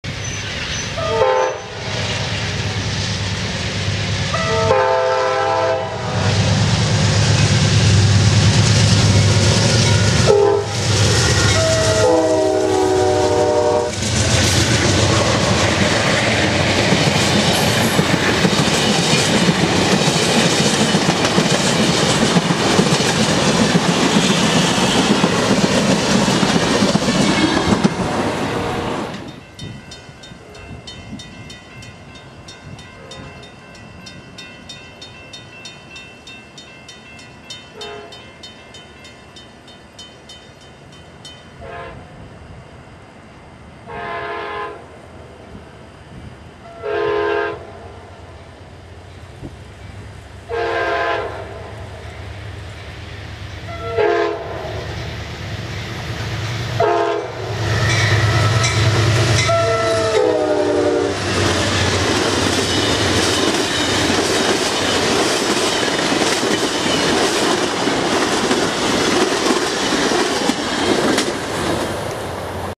Railfanning 10/19/09: P092 at Winston Wye (Inbound and Outbound)
Amtrak 92 is seen twice is this clip as he races out of Lakeland in the first shot and then out of Tampa in the second one.